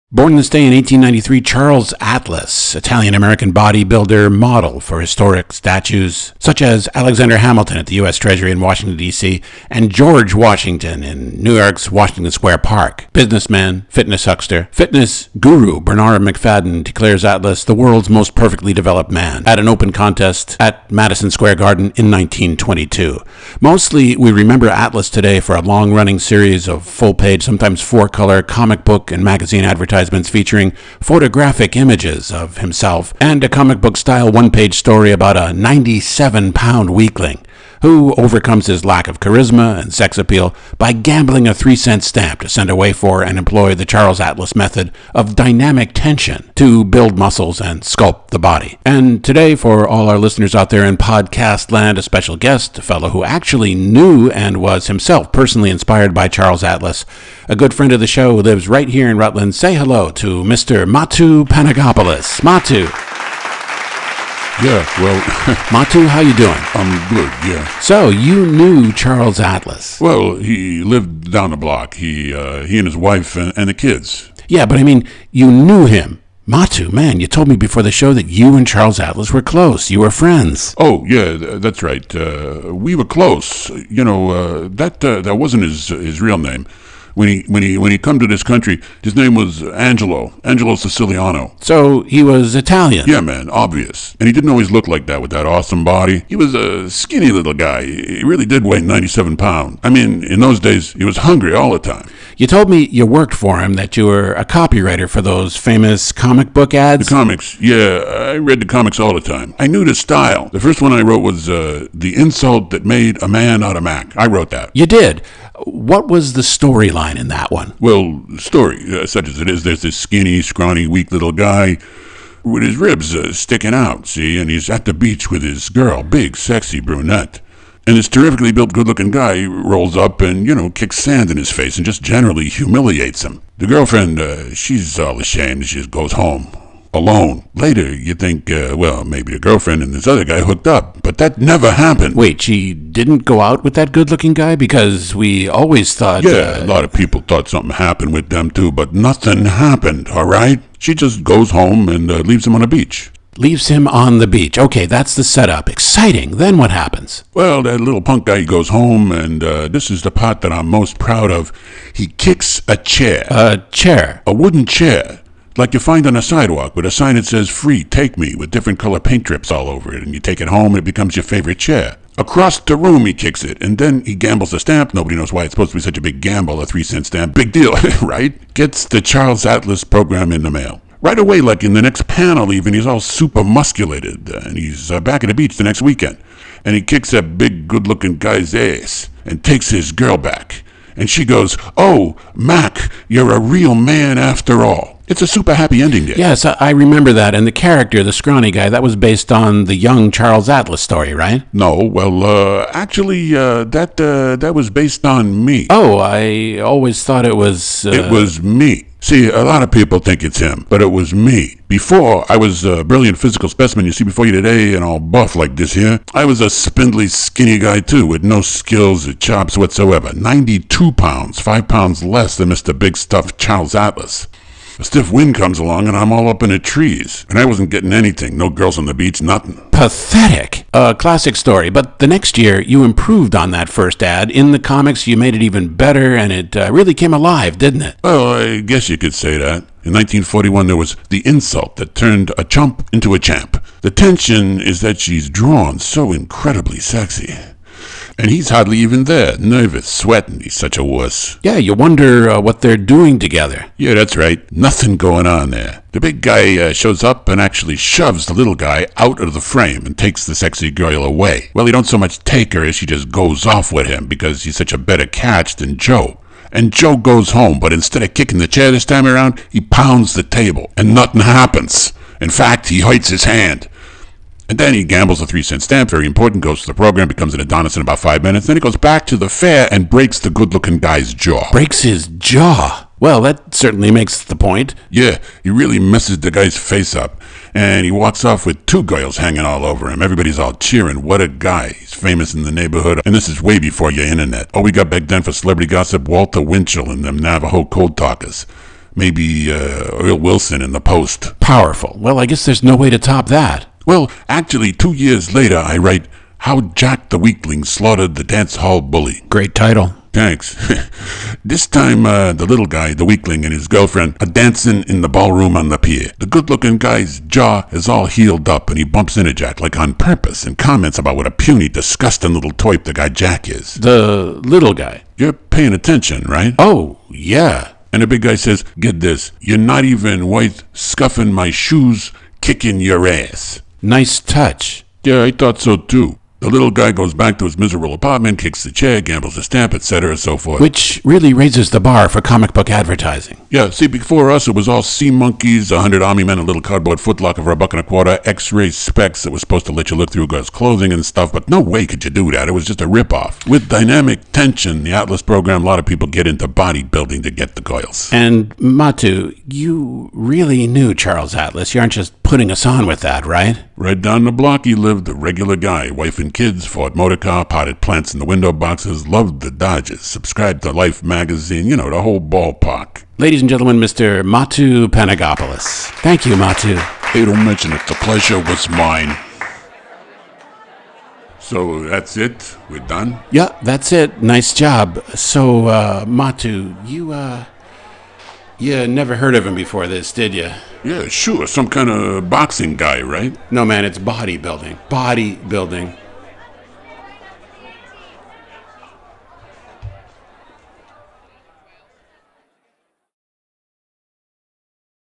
THE MAN WHO KNEW CHARLES ATLAS
RICH ALCOTT interviews MATOU PANAGOPOULOUS about his work with body building, businessman, fitness huckster, ‘the world’s most perfectly developed man,’ CHARLES ATLAS, as a copy writer of the famous series of comic book advertisements for ‘Dynamic Tension,’ a system Atlas himself developed for building muscles and body sculpting.